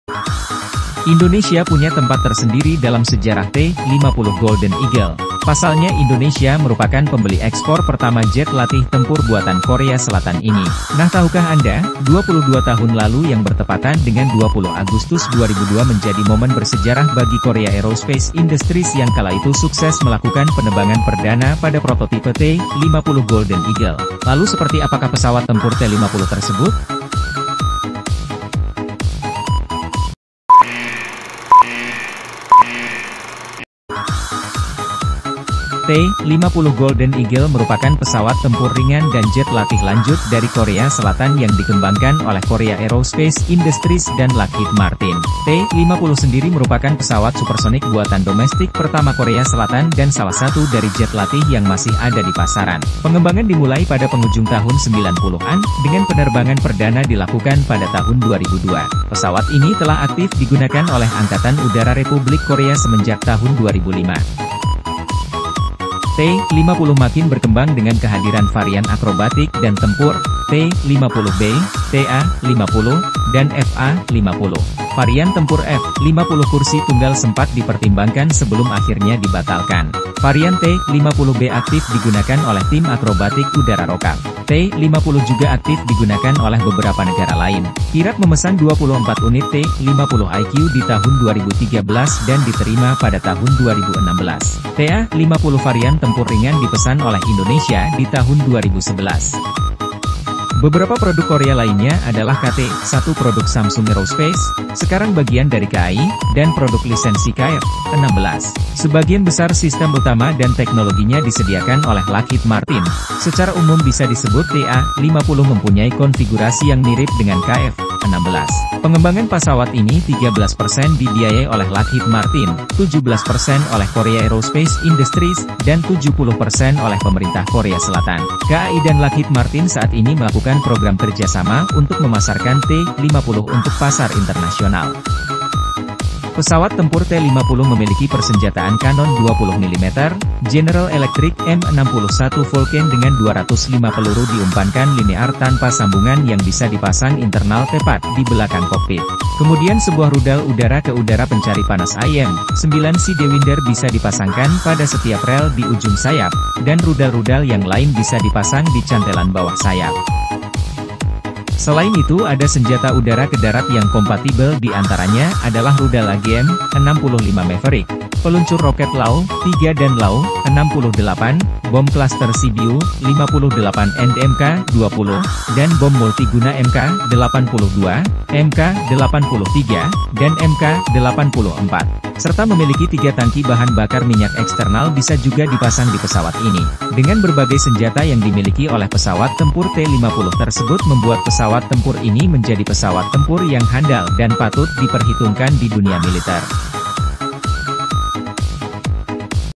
PESAWAT TEMPUR TERCANGGIH BUATAN KORSEL//SOUTH KOREA'S MOST ADVANCED FIGHTER PLANE
Indonesia punya tempat tersendiri dalam sejarah T-50 Golden Eagle, pasalnya Indonesia merupakan pembeli ekspor pertama jet latih tempur buatan Korea Selatan ini. Nah tahukah Anda, 22 tahun lalu yang bertepatan dengan 20 Agustus 2002 menjadi momen bersejarah bagi Korea Aerospace Industries yang kala itu sukses melakukan penebangan perdana pada prototipe T-50 Golden Eagle.
Lalu seperti apakah pesawat tempur T 50 tersebut?

T-50 Golden Eagle merupakan pesawat tempur ringan dan jet latih lanjut dari Korea Selatan yang dikembangkan oleh Korea Aerospace Industries  dan Lockheed Martin.

T-50 sendiri merupakan pesawat supersonik buatan domestik pertama Korea Selatan dan salah satu dari jet latih yang masih ada di pasaran.
Pengembangan dimulai pada penghujung tahun 90an, dengan penerbangan perdana dilakukan pada tahun 2002. Pesawat ini telah aktif digunakan oleh Angkatan Udara Republik Korea semenjak tahun 2005.

Pesawat tempur T 50 memiliki persenjataan Kanon 20 mm, General Electric M61 Vulcan dengan 205 peluru diumpankan linear tanpa sambungan yang bisa dipasang internal tepat di belakang kokpit.
Kemudian Sebuah rudal udara ke udara pencari panas AIM-9 Sidewinder bisa dipasangkan pada setiap rel di ujung sayap, dan rudal-rudal yang lain bisa dipasang di cantelan bawah sayap.

Selain itu ada Senjata udara ke darat yang kompatibel di antaranya adalah rudal AGM-65 Maverick, peluncur roket LAU-3 dan LAU-68, bom kluster CBU-58 and Mk-20, dan bom multiguna Mk-82, Mk-83, dan Mk-84 .
Serta memiliki Tiga tangki bahan bakar minyak eksternal bisa juga dipasang dipesawat ini.

Dengan berbagai senjata yang di miliki oleh pesawat tempur T 50 tersebut membuat pesawat tempur ini menjadi pesawat tempur yang handal dan patut diperhitungkan di dunia militer.

Indonesia has a special place in the history of the T-50 Golden Eagle, because Indonesia is the first export buyer of this South Korean-made fighter jet trainer. Well, did you know, 22 years ago, which coincided with August 20, 2002, was a historic moment for Korea Aerospace Industries, which at that time successfully carried out the first flight of the T-50 Golden Eagle prototype.
So what is the T 50 fighter jet like?

The T-50 Golden Eagle is a light fighter and advanced jet trainer from South Korea developed by Korea Aerospace Industries and Lockheed Martin.

The T-50 itself is South Korea's first domestically made supersonic aircraft and one of the jet trainers still on the market.

 Development began in the late 90s, with the first flight in 2002. This aircraft has been actively used by the Republic of Korea Air Force since 2005.

The T 50 fighter aircraft has a 20 mm cannon armament, General Electric M61 Vulcan with 205 rounds fed linearly without a connection that can be installed internally just behind the cockpit.

Then an AIM-9 Sidewinder heat-seeking air-to-air missile can be mounted on each rail at the wingtip, and other missiles can be mounted on the underwing pylons.

In addition, there are compatible air-to-ground weapons including the AGM-65 Maverick missile, LAU-3 and LAU-68 rocket launchers, CBU-58 and Mk-20 cluster bombs, and Mk-82, Mk-83, and Mk-84 multipurpose bombs.
And has Three external fuel tanks can also be installed on this aircraft.

 With the various weapons owned by the T 50 fighter plane, this fighter plane has become a reliable fighter plane and is worthy of being reckoned with in the military world.